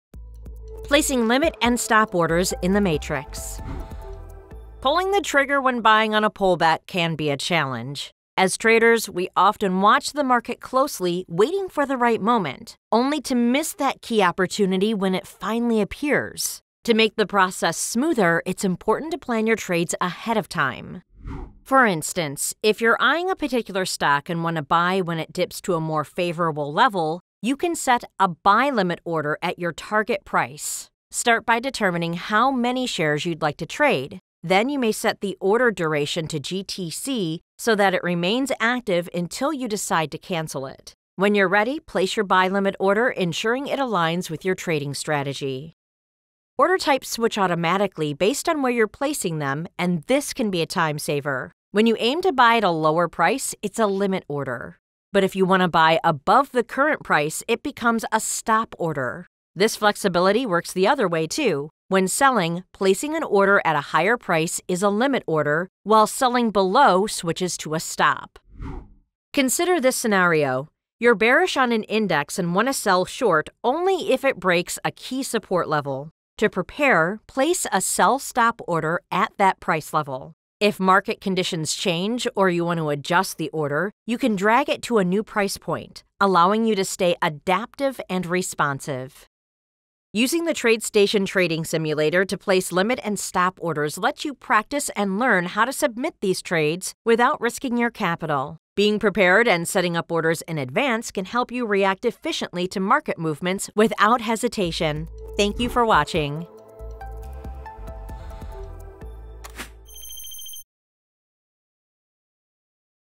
Learn Tradestation - Placing Limit and Stop Orders in the Matrix
Use the Matrix to set pullback/breakout orders, adapt and adjust seamlessly, and practice risk-free using TradeStation trading simulator.